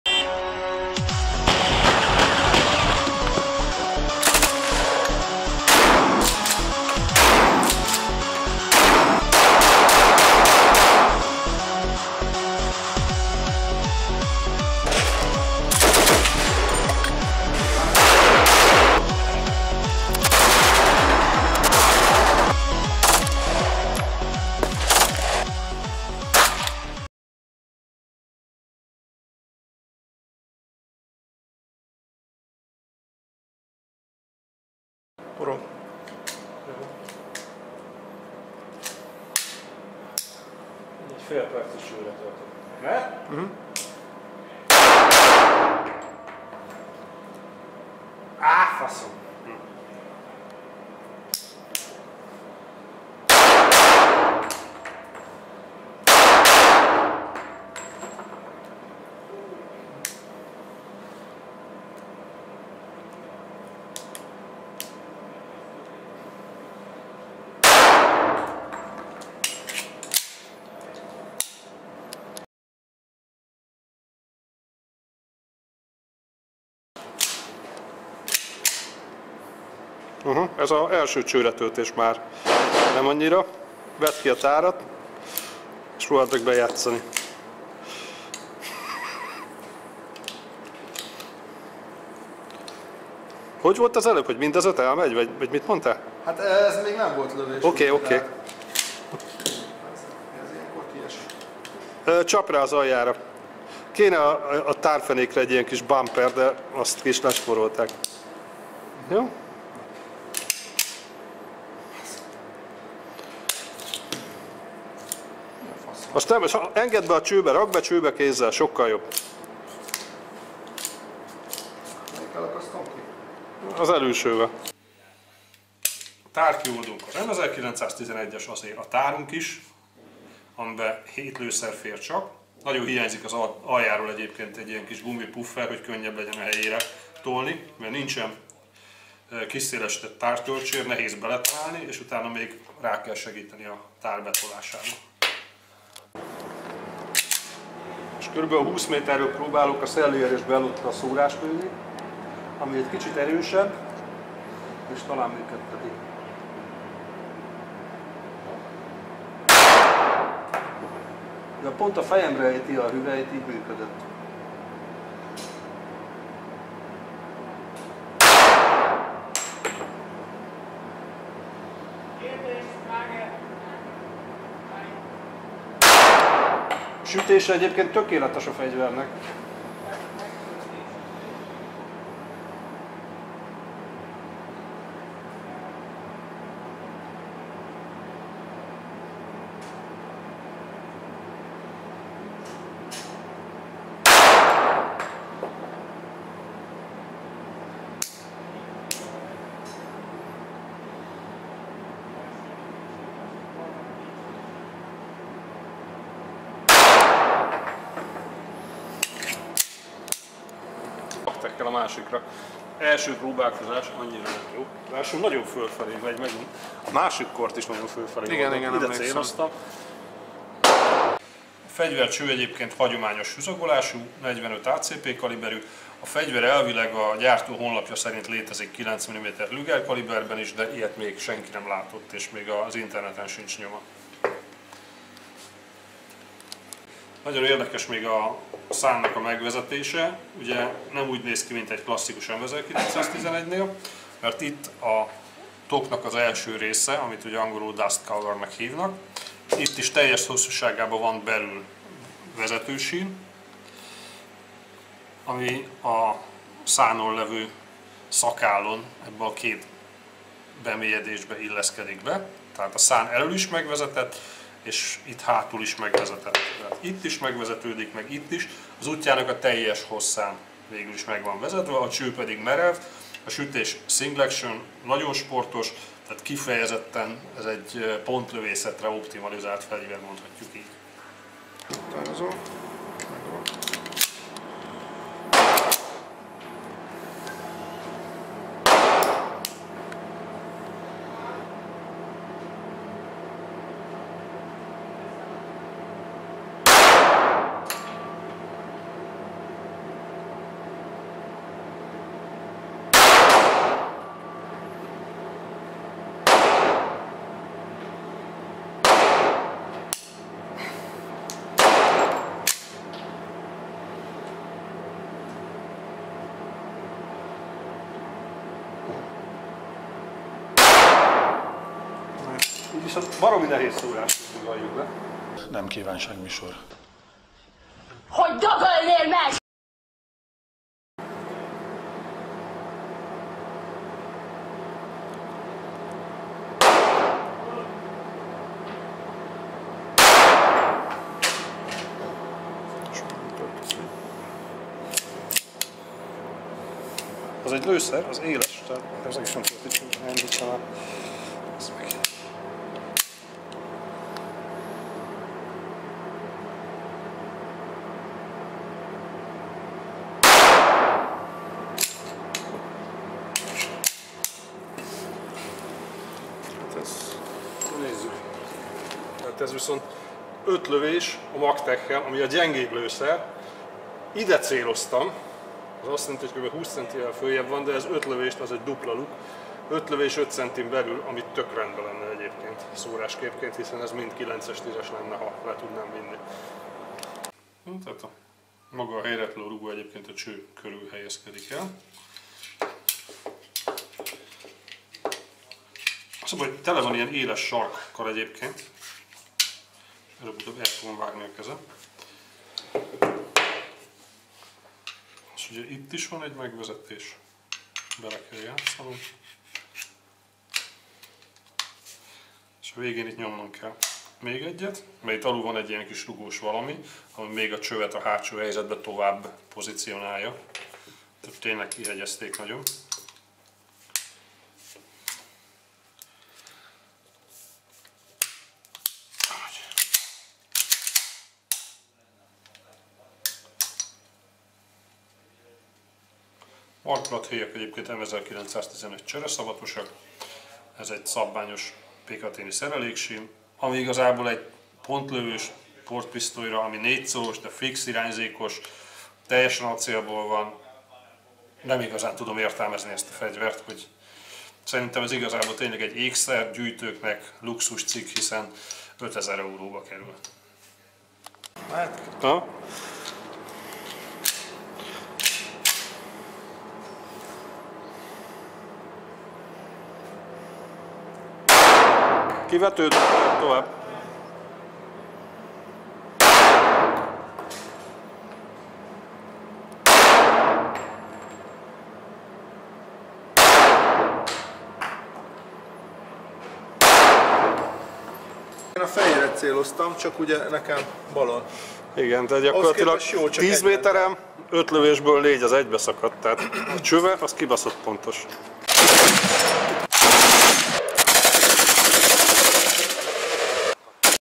Korth PRS .45 ACP 2. rész
Folytatjuk a 2016 novemberi Kaliberben is kitárgyalt Korth PRS luxuspisztoly bemutatását. A görgős késleltetésű, merev csövű, duplán is megvezetett szános pisztoly tesztje azonban igazi kínszenvedés volt, bár ebben a részben már lesz olyan is, hogy 5 lövés kimegy belőle akadály nélkül. Igaz, olyan is, hogy már csőretöltéskor elakad, és 5 lövés leadása majdnem 30 másodpercig tart.
Lőszerköltségre érzékenyek nagy kedvence lehet:-)))